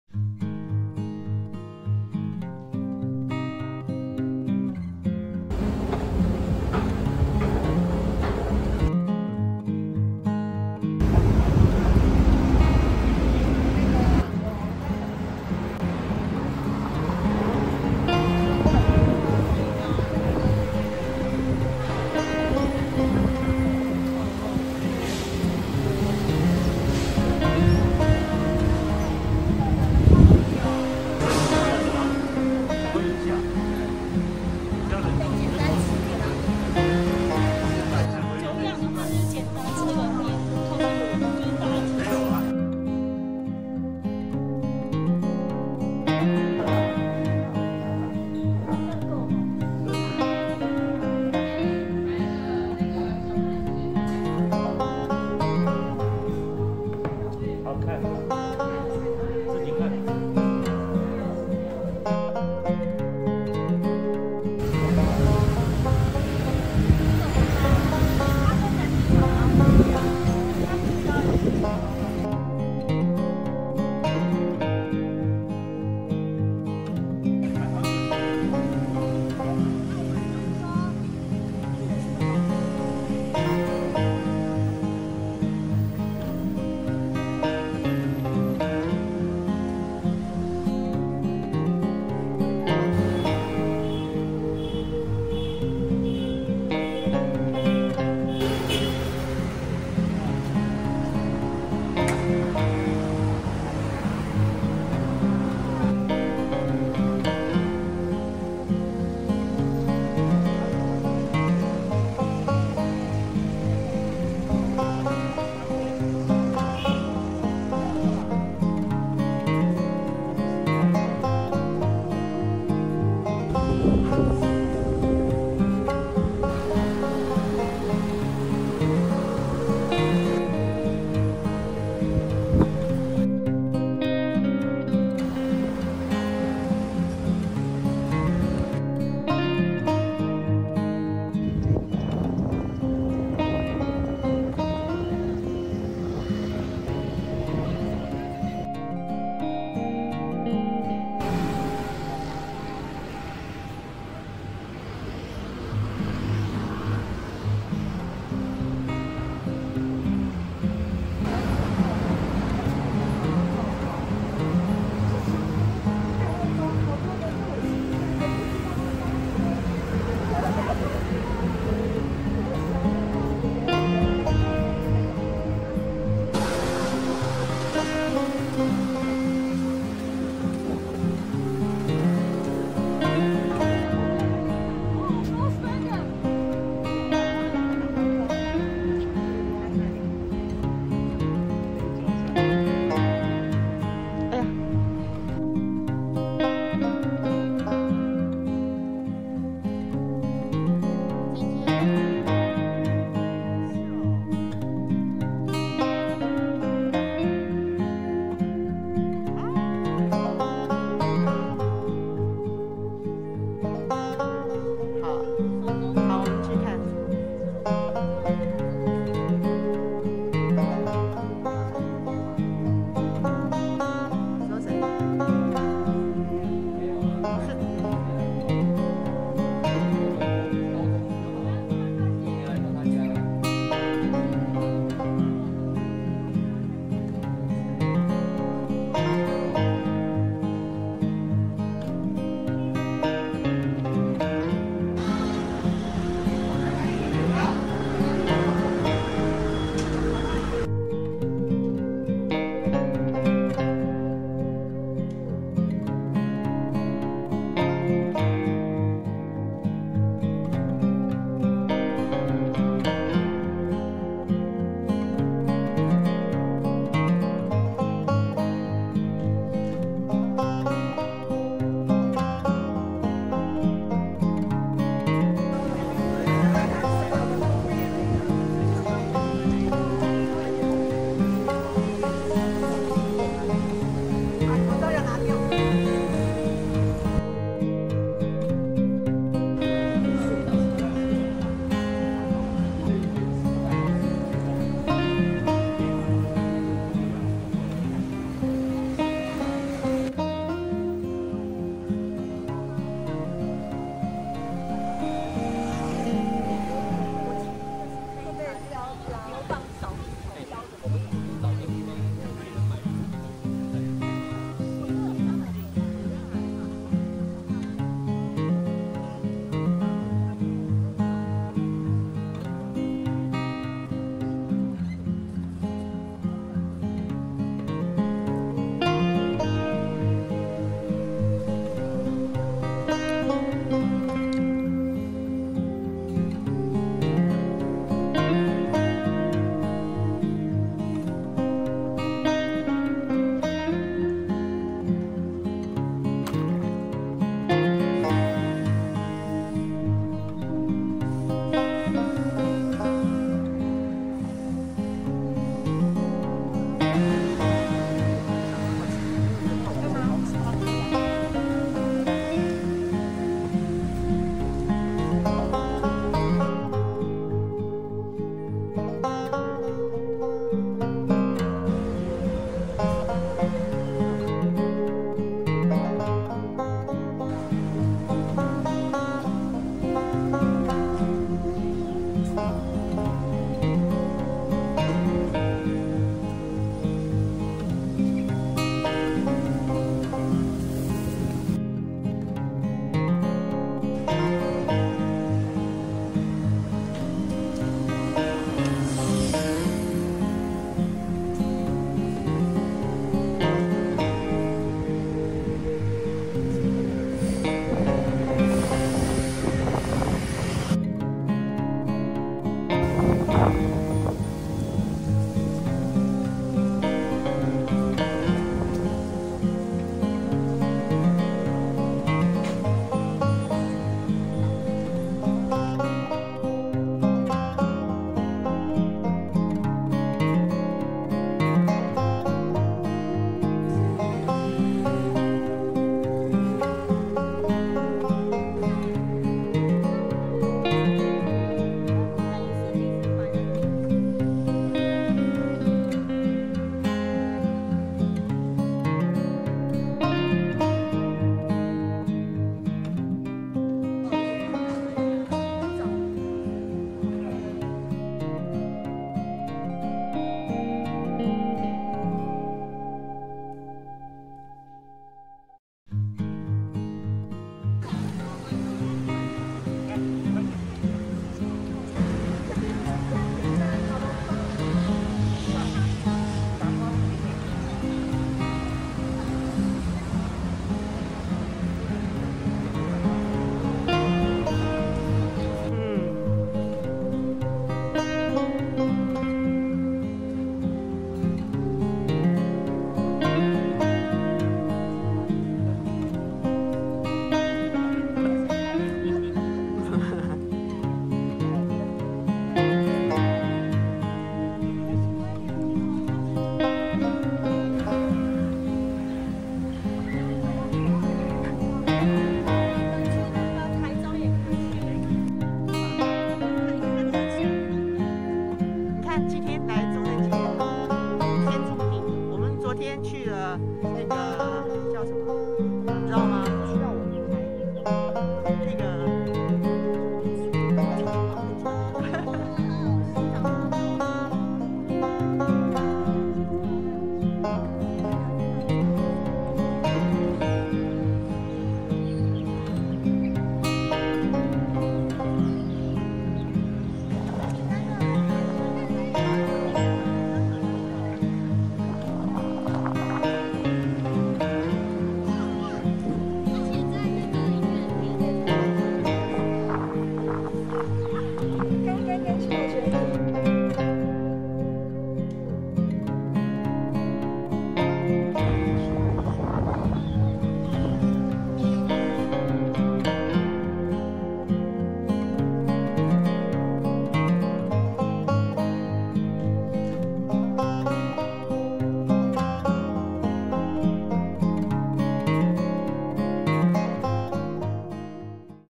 榕錦時光小散步| 永康街-金華街榕錦時光-中正紀念堂
榕錦時光小散步

捷運東門站5號出口
永康街 誠記 越式料理
永康街 昔慕思芒果冰
金華街 台北聖殿教堂
金華街 榕錦時光生活園區
中正紀念堂
春水堂 珍珠奶茶

榕錦時光生活園區原為台北刑務所官舍，建於日據時期1895年，九連棟的建築，曾經遭祝融侵襲，透過臺北市文化局「老房子文化運動計畫」下，老屋新生的修復、改建，重現老屋樣貌。

園區內有一排舊圍牆，訴說著市定古蹟「臺北監獄圍牆遺蹟」的歷史，如何活化老屋而不進行破壞，找回昔日風采，創造古今交融。